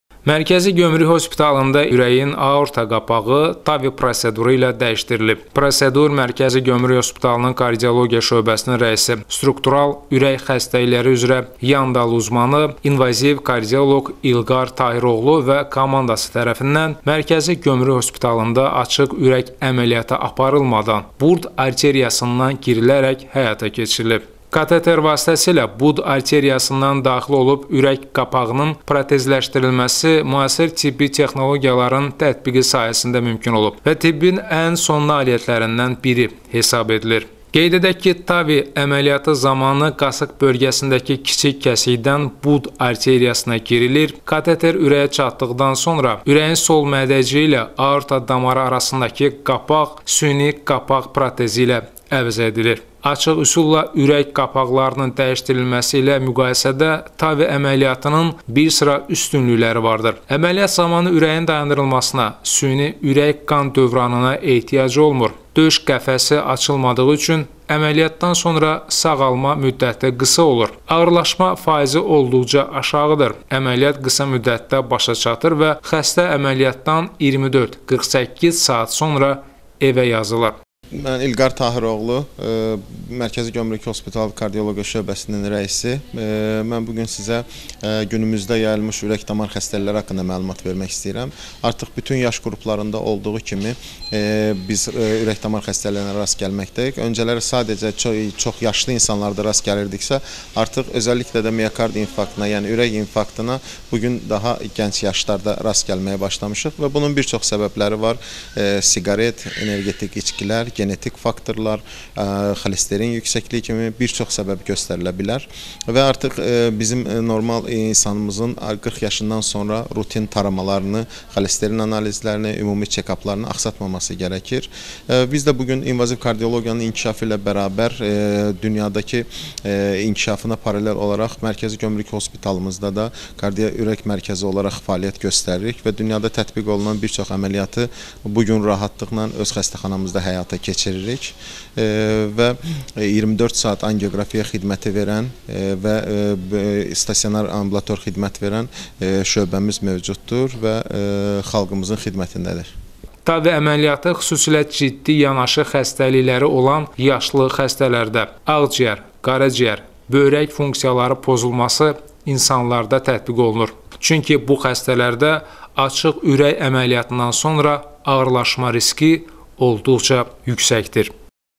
Dr.İlqar Tahiroğlu və komandası TAVI əməliyyatı.
Mərkəzi Gömrük Hospitalının Kardiologiya şöbəsinin rəisi, struktural ürək xəstəlikləri üzrə yandal uzmanı, invaziv kardioloq Dr.İlqar Tahiroğlu və komandası tərəfindən Mərkəzi Gömrük Hospitalında açıq ürək əməliyyatı aparılmadan, TAVI əməliyyatı vasitəsilə bud arteriyasından girilərək ürəyin aorta qapağının dəyişdirilməsi əməliyyatının həyata keçirilməsi barədə “SƏS TV”-də yayımlanan süjeti Sizə təqdim edirik.

🏨 Bakı, K.Kazımzadə, 118